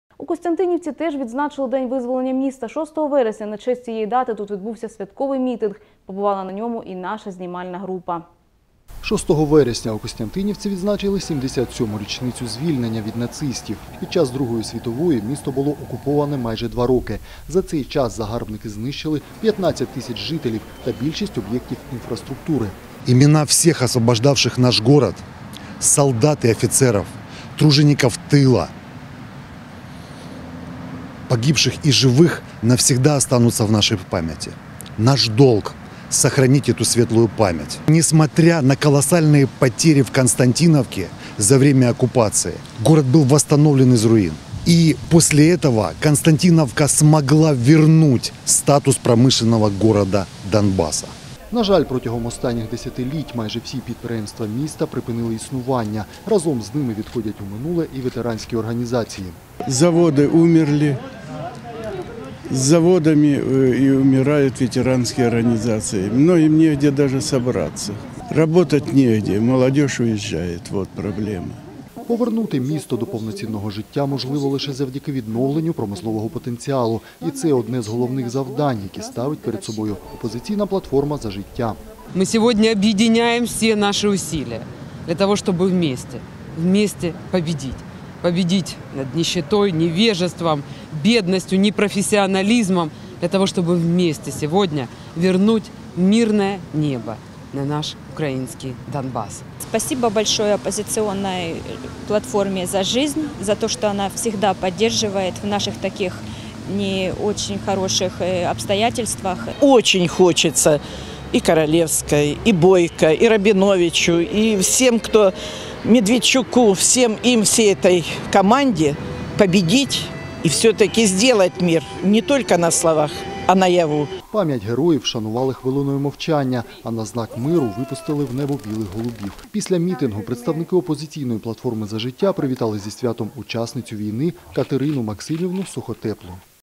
У Костянтинівці теж відзначили День визволення міста.
У Костянтинівці теж відзначили День визволення міста. Шостого вересня на честь цієї дати тут відбувся святковий мітинг. Побувала на ньому і наша знімальна група.